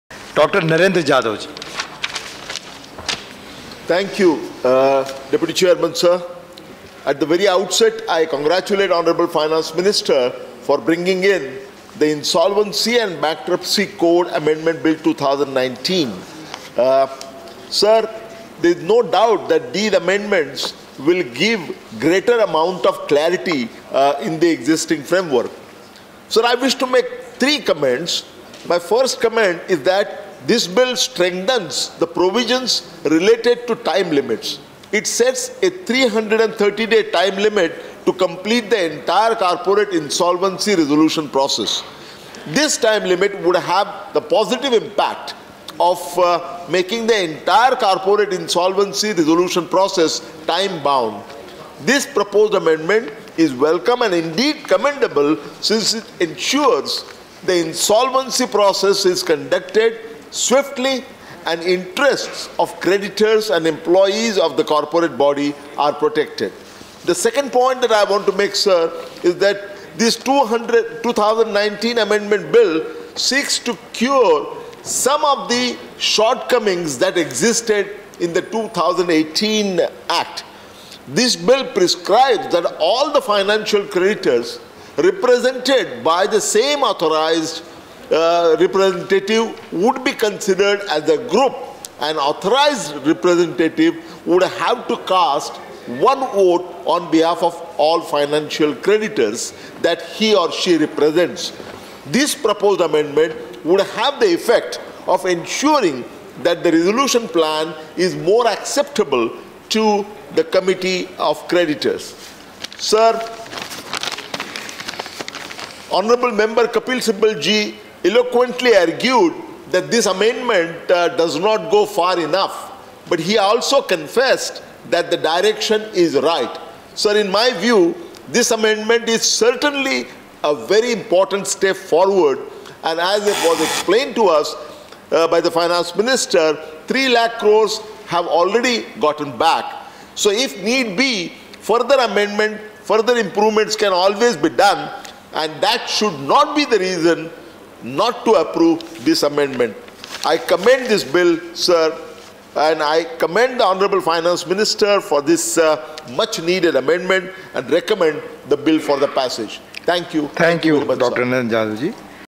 Dr Narendra Jadhav (2019) Parliament (Rajya Sabha) Intervention on Insolvency & Bankruptcy Code 2019
Dr Narendra Jadhav (2019) Parliament (Rajya Sabha) Intervention on Insolvency and Bankruptcy Code Amendment Bill,  2019